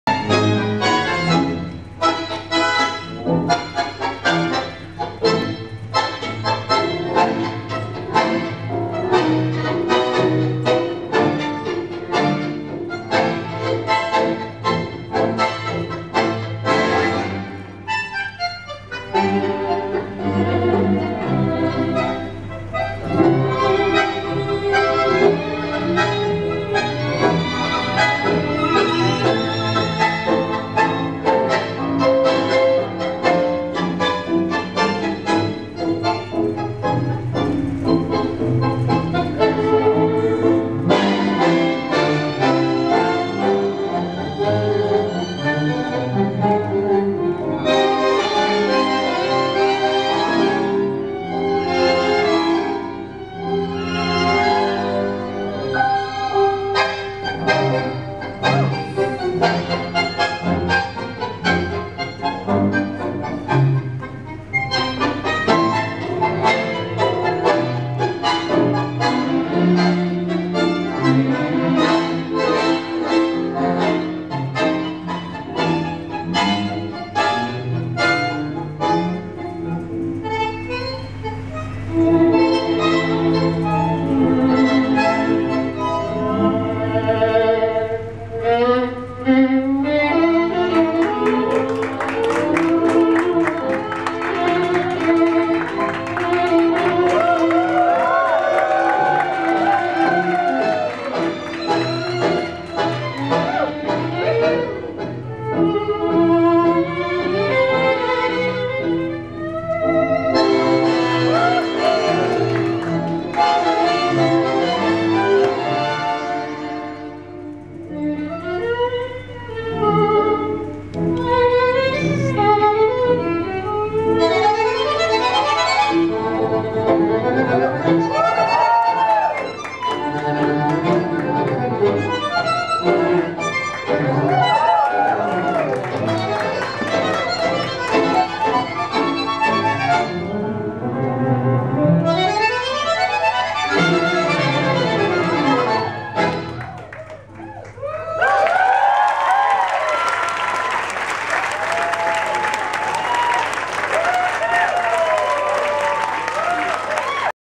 Leandro Oliver & Laila Rezk Milonga por Dos Hong Kong October 19th 2013 FIRST DANCE
Historic night in Hong Kong October 19th 2013 the first-ever Double-bill outside of Argentina featuring two stellar couples and friends Gabriel MIsse & Analia Centurion AND Leandro Oliver & Laila Rezk.  They performed 5 unforgettable dances in a first-of-its-kind milonga organized by Ray Rudowski with cooperation from Raymond Chu & Lily Cheng.   The couples performed 5 dances starting with Laila & Leandro, this is the FIRST dance in the sequence performed by Laila & Leandro.